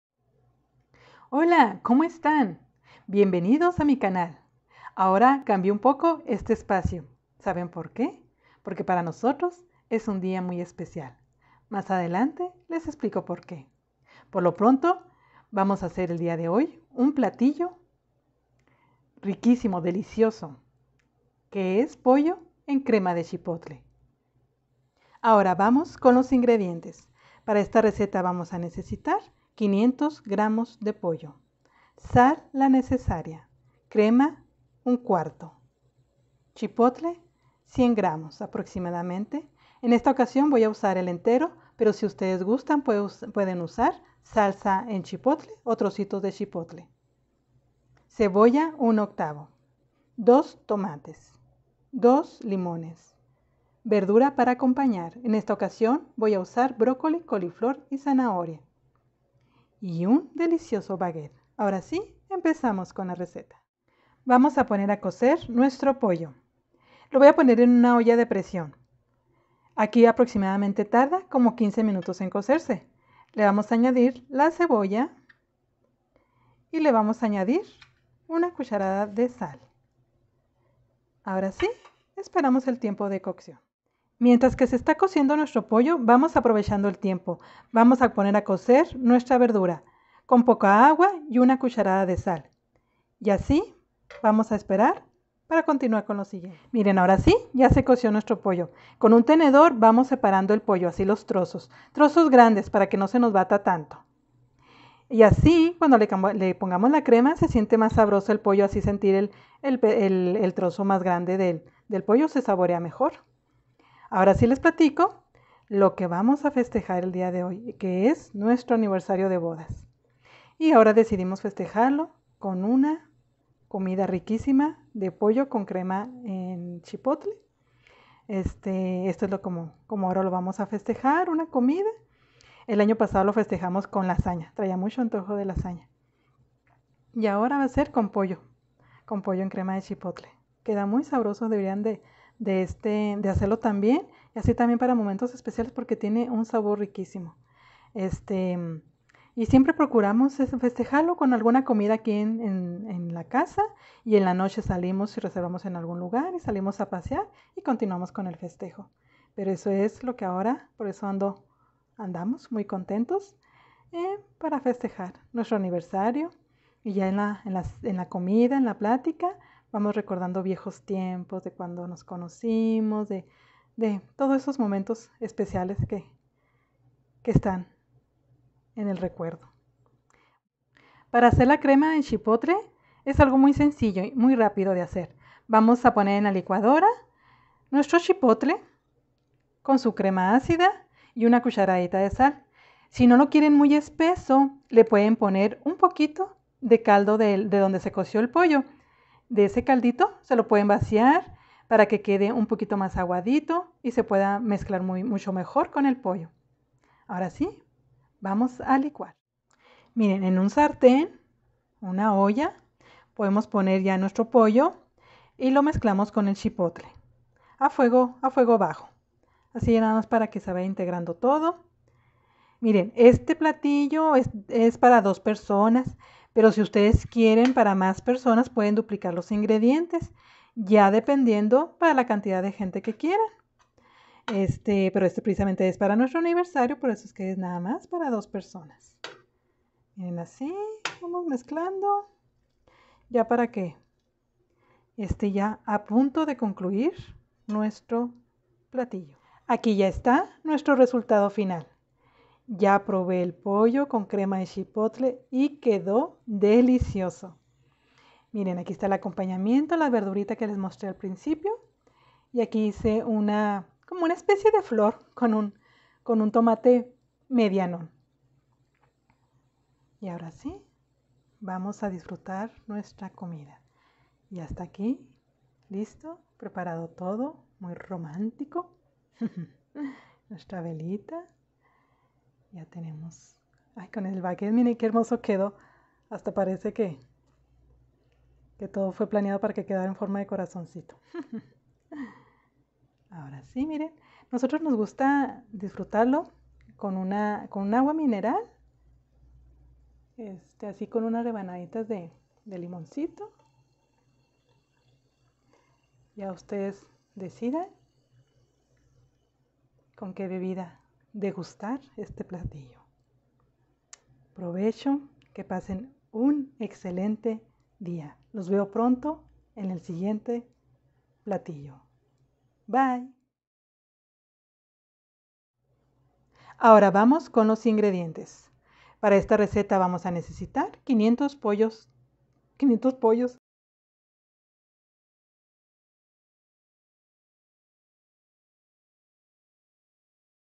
POLLO EN CREMA DE CHIPOTLE FÁCIL Y RÁPIDO ❣️DE ANTOJO EN NUESTRO ANIVERSARIO❣️ + BLOOPERS 😅
COMO HACER POLLO EN CREMA DE CHIPOTLE

Ingredientes:

* 500gr de pollo
* Crema 1/4
* Chipotle entero 100gr
* Cebolla 1/8
* 2 tomates
* 2 limones para decorar
* Verduras al gusto
* Sal
* Un baguette

     Nosotros podemos hacer de un platillo especial para festejar cualquier evento de nuestra vida, poniéndole un toque decorativo y mucho amor ❤️
     Espero que ésta idea les pueda ayudar para sus momentos especiales 😘